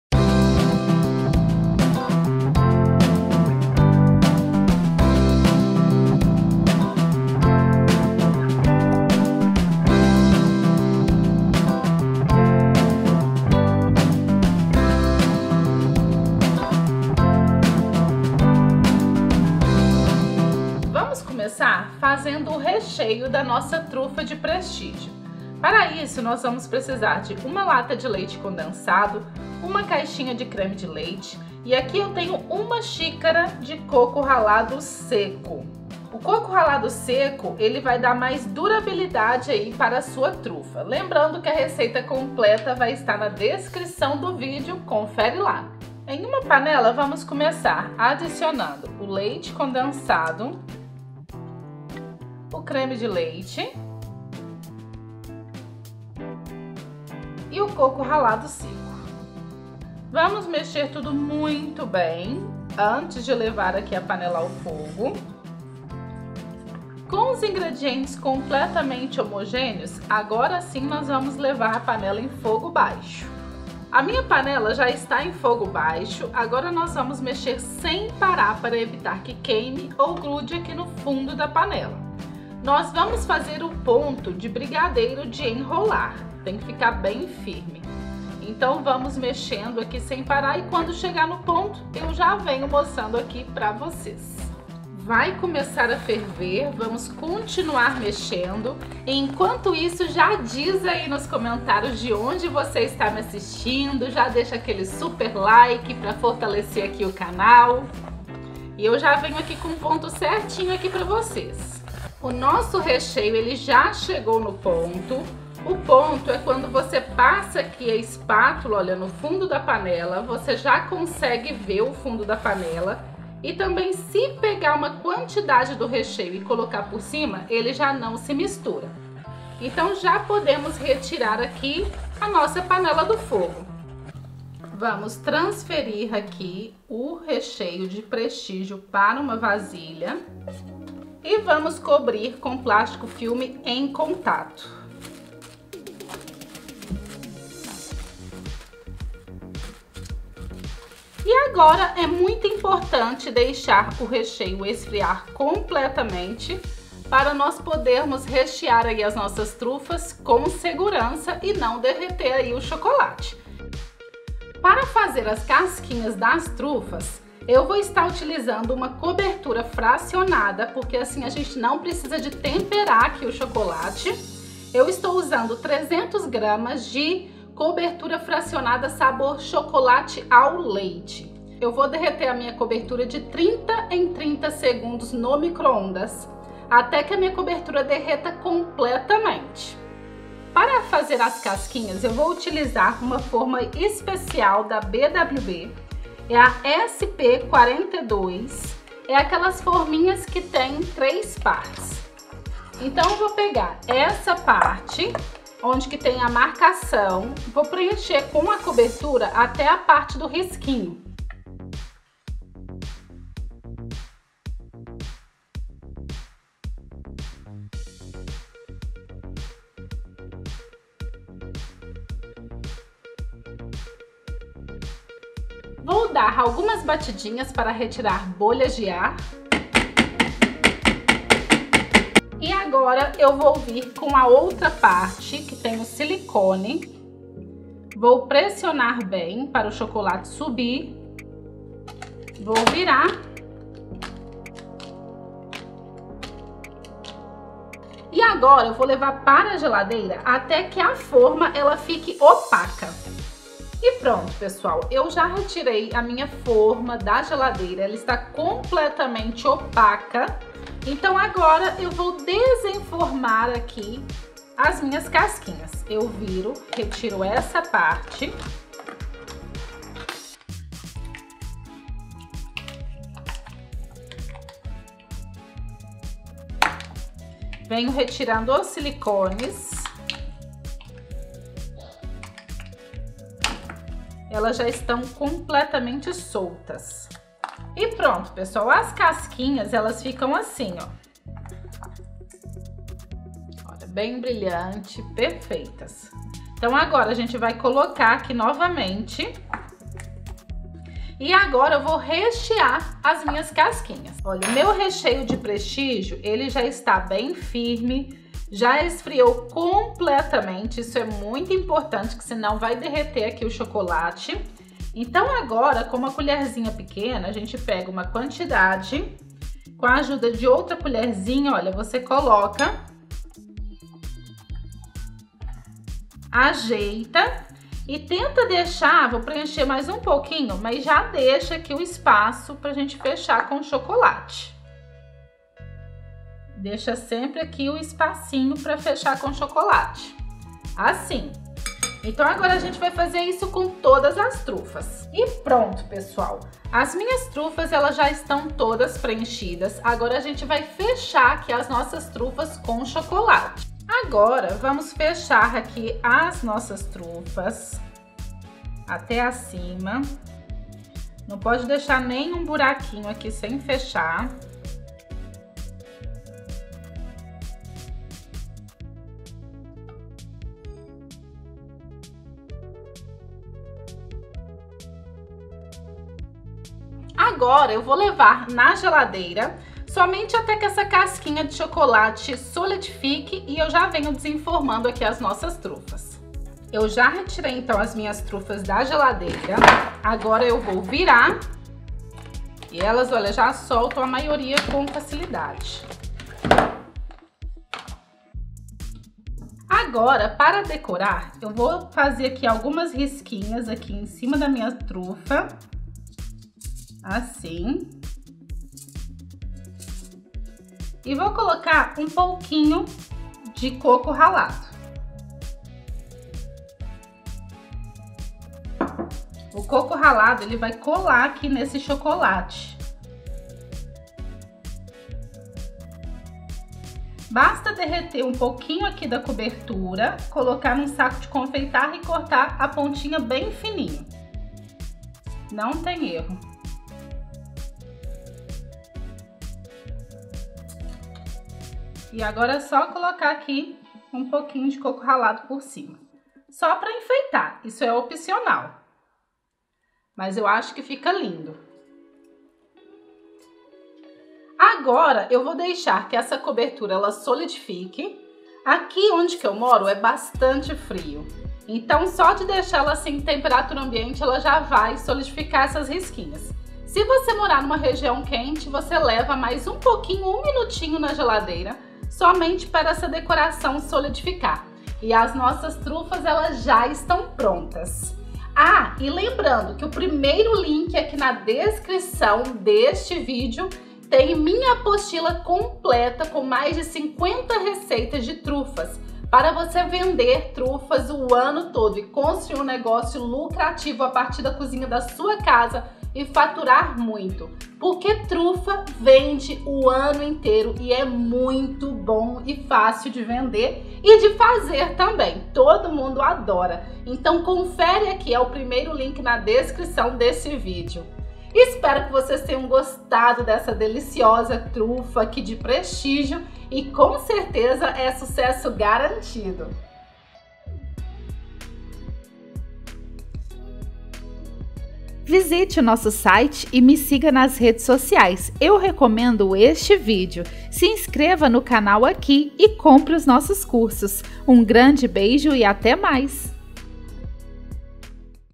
TRUFA DE PRESTÍGIO - FAÇA E VENDA TRUFAS DE CHOCOLATE
Aprenda a fazer deliciosas trufas de prestígio para vender. Esta trufa de chocolate sabor prestígio é ótima para vender e faturar uma graninha extra a partir de casa. Espero que goste!

OBS:  Aminha xícara é de 240ml

RECEITA DA TRUFA DE PRESTÍGIO:
1 lata de leite condensado
1 caixa de creme de leite
1 xícara de coco ralado seco
300g de cobertura sabor chocolate ao leite